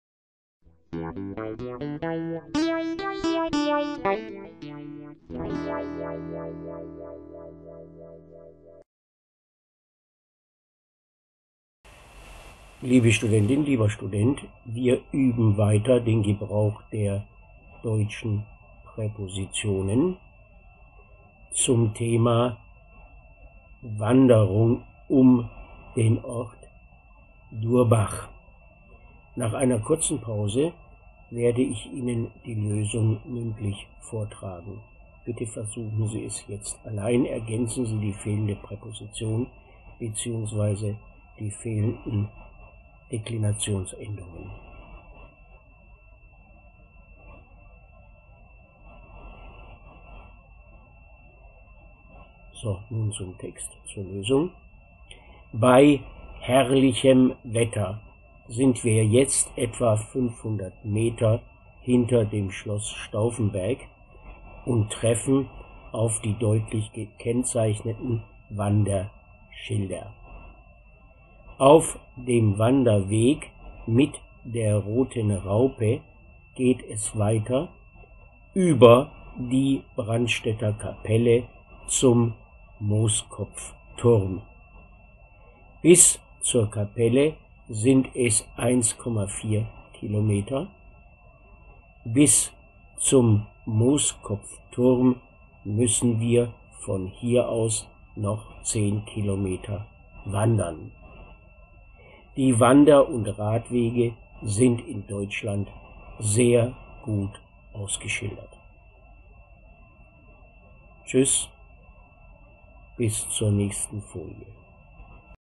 A1. Kap.03 Übung 17 Grammatik Präpositionen Rundwanderung Durbach
Wir üben den Gebrauch der deutschen Präpositionen.
Lösung: Bei, -m / hinter dem / auf / Auf dem / mit der / über die / zum / Bis zur / bis zum / von hier aus in
Ein schwieriges Kapitel, da die Präpositionen sehr zahlreich und der Gebrauch nicht immer logisch ist. Siehe auch unsere Grammatik-Tafeln zu Kap. 03 und unsere Grammatik Videos auf youtube zieschekolleg, ziescheelearning oder ziesche-elearning.de